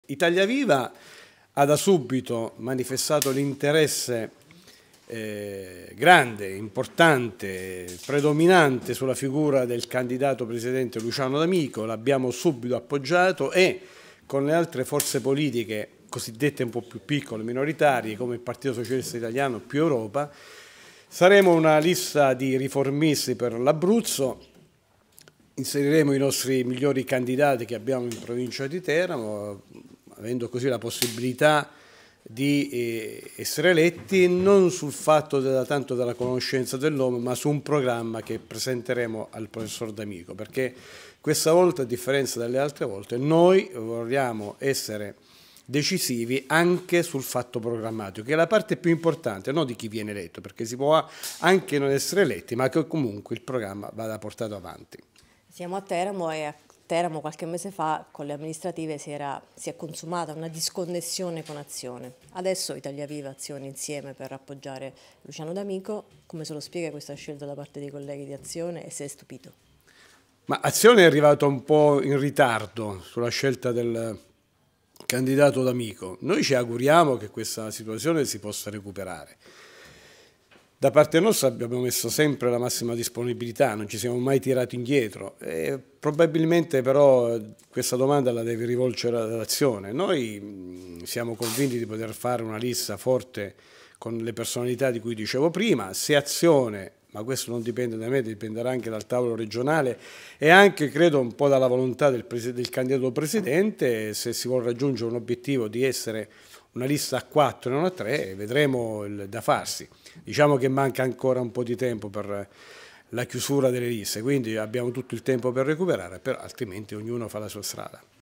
Monticelli: "Il ritorno di Azione? Tardivo, speriamo di ricomporre"
Il candidato presidente provinciale di Italia Viva parla dell'appoggio a Luciano D'Amico e non solo